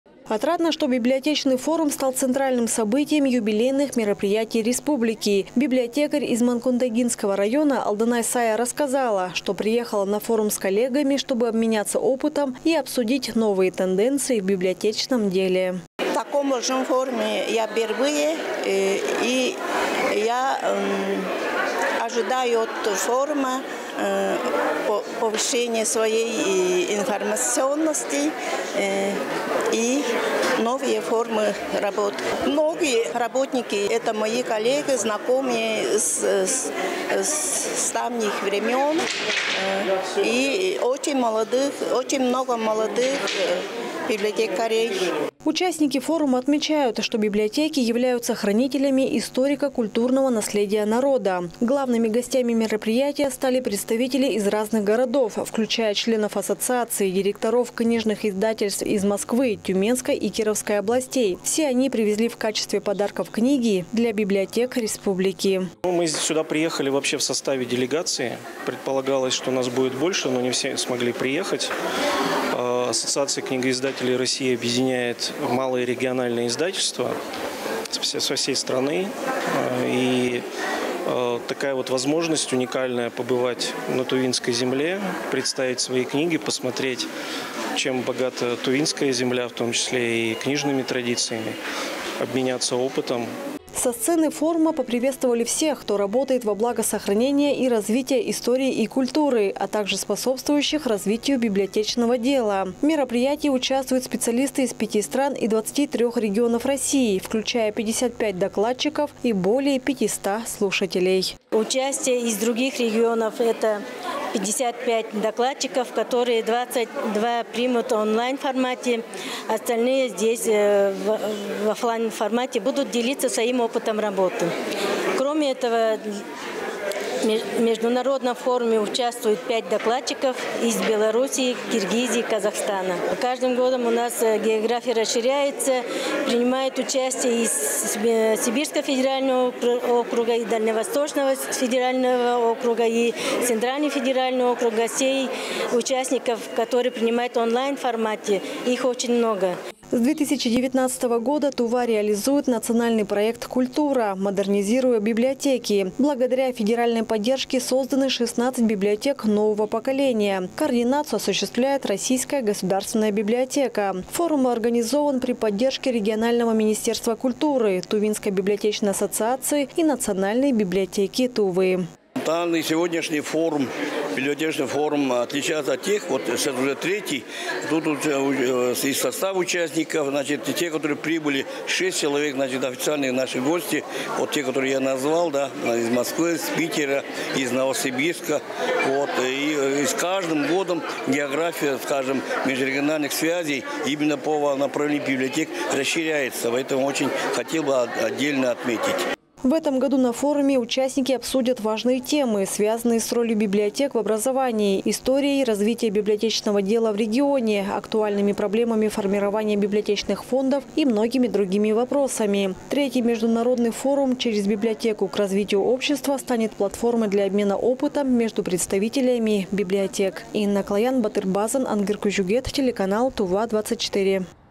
В Туве стартовал III Международный форум «Через библиотеку - к развитию общества»
Жители республики сегодня и завтра смогут принять активное участие в III Международном форуме «Через библиотеку – к развитию общества», посвященном 80-летию добровольного вхождения ТНР в состав России. Форум собрал более 55 докладчиков, представляющих различные уголки России, а также из некоторых зарубежных стран. О количестве участников и главных гостях мероприятия расскажет Инна Клоян.